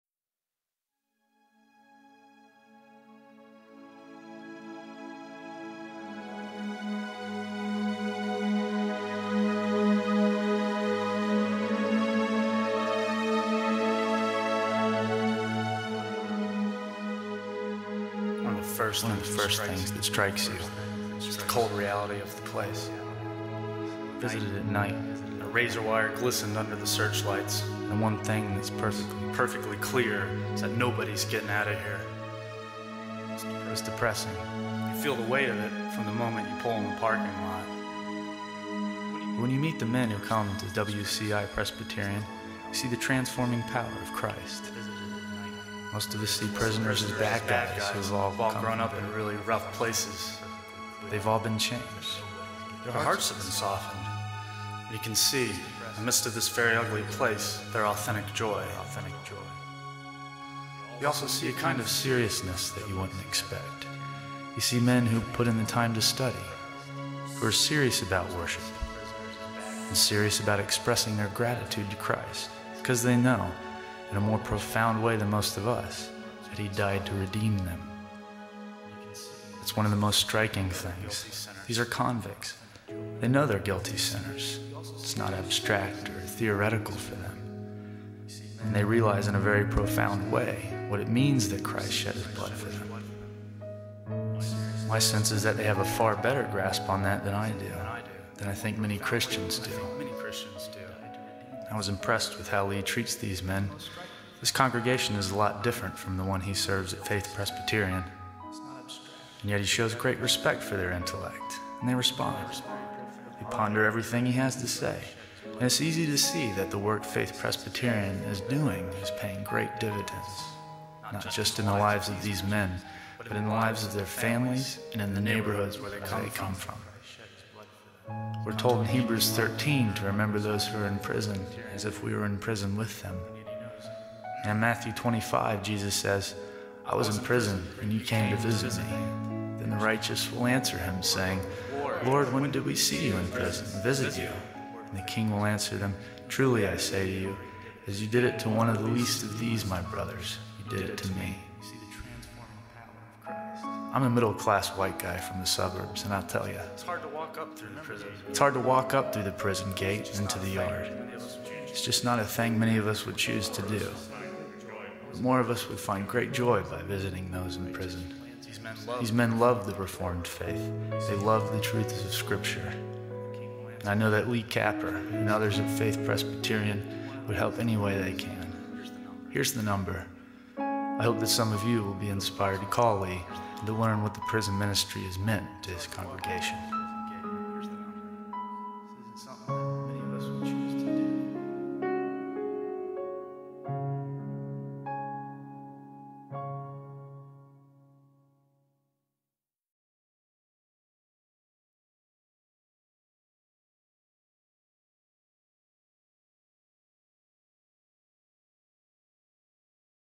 Peace and Purpose Behind Bars
A video produced in Photoshop and Aftereffects for the PCA website.  Copy written by Richard A. Doster.  Photography by Ted Haddock.  Voiceover, soundtrack, and compilation by Robert William Haddock the First.